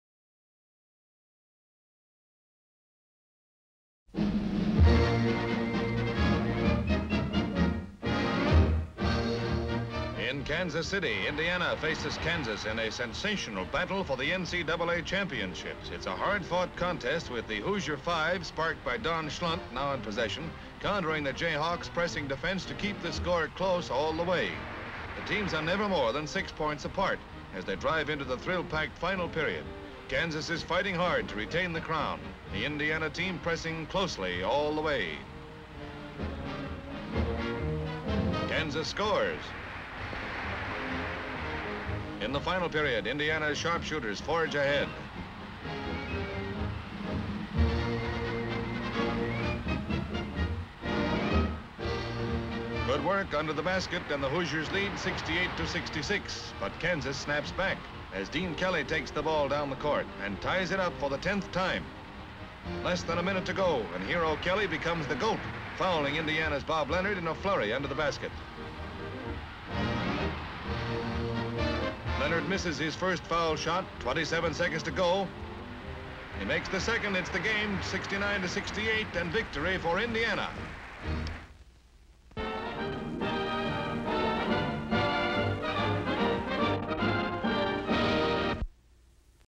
Indiana wins a basketball match against Kansas held in Kansas, United States . HD Stock Footage
Indiana wins a basketball match against Kansas held in Kansas, United States .

A basketball match between Indiana and Kansas for National Collegiate Athletic Association (NCAA) crown being held in Kansas, United States . A crowd in a stadium. A banner reads: 'Kansas NCAA Champs'. The match in progress. The crowd cheers and applauds. Indiana wins the match. Location: Kansas United States. Date: March 19, 1953.

57,000+ broadcast-quality historic clips for immediate download.